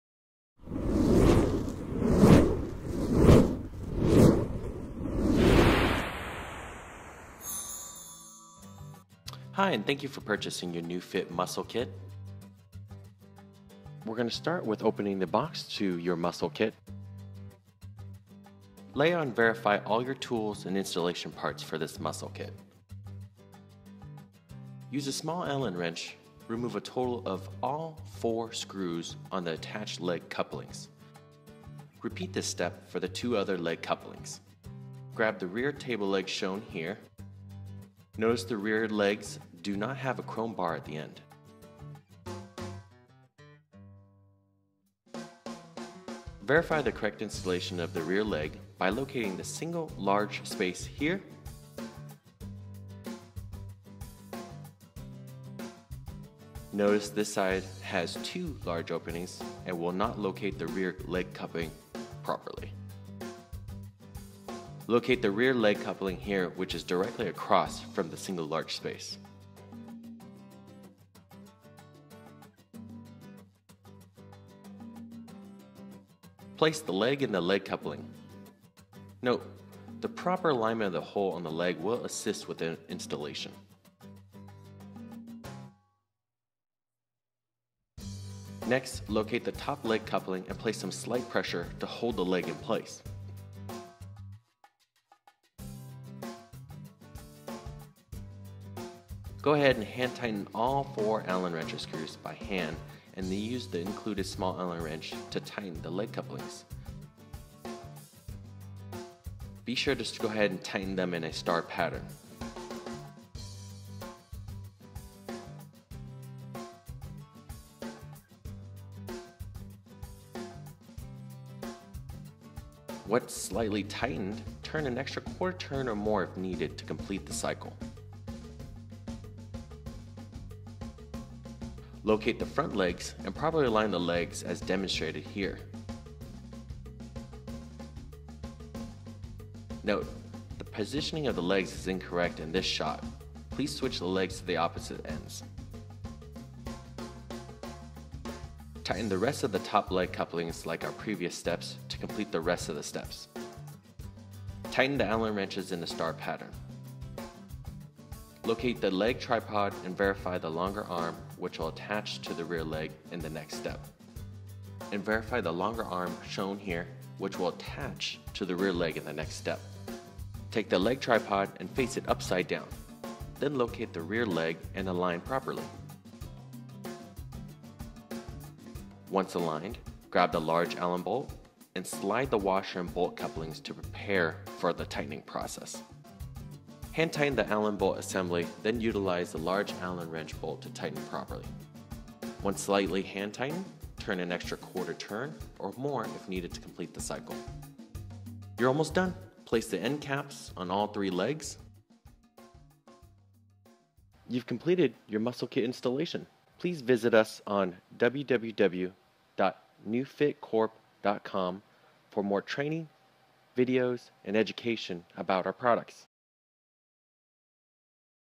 NuGym Muscle Kit Installation Simple Install - Fitness Direct
Follow this simple and easy video on how to install the NuGym Muscle Kit. The tools and installation process takes no longer then 15 minutes to setup.